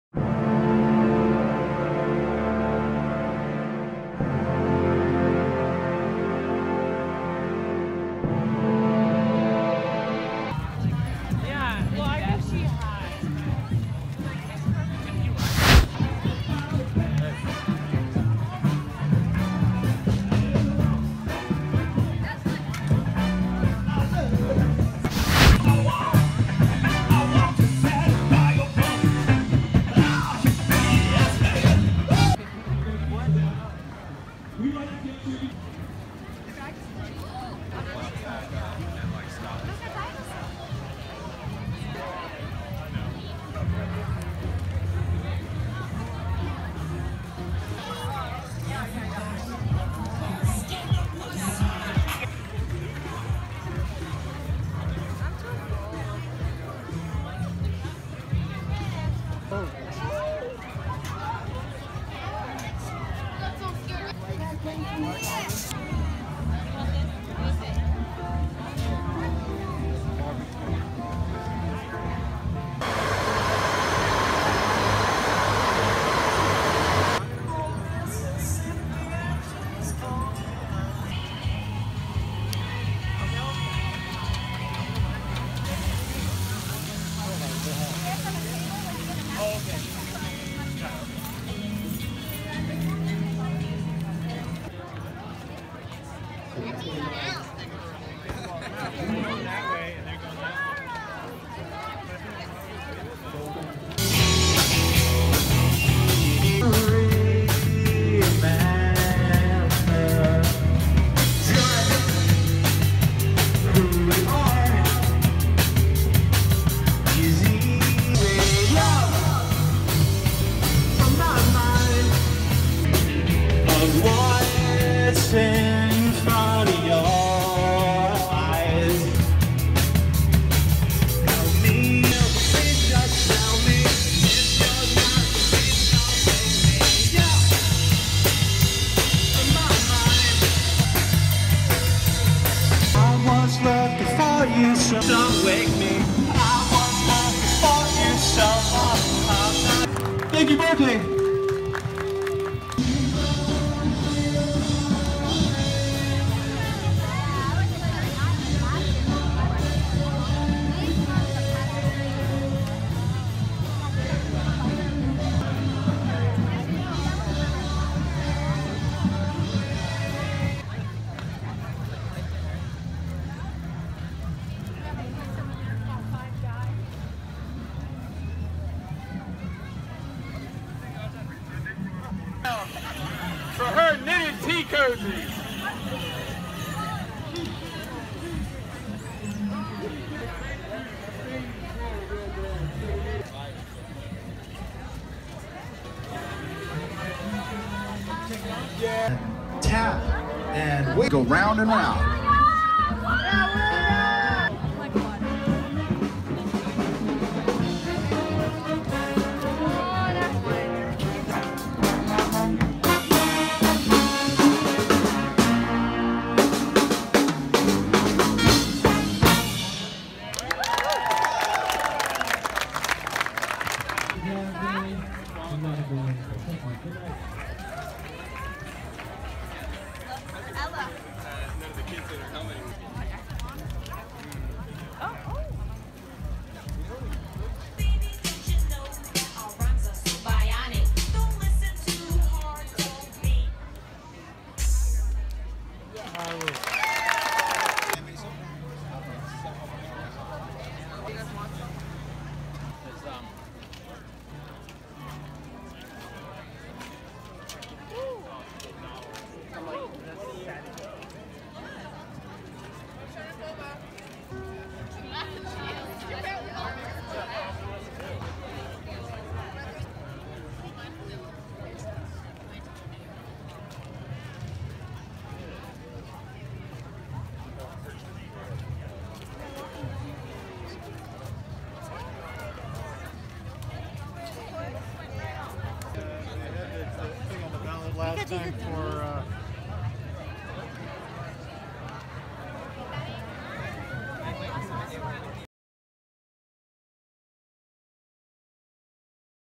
Solano Avenue Stroll Berkley.Ca
Since 1974 Solano Avenue and the twin-cities of Albany and Berkeley CA, have hosted the Solano Avenue Stroll, the East Bay’s largest street festival!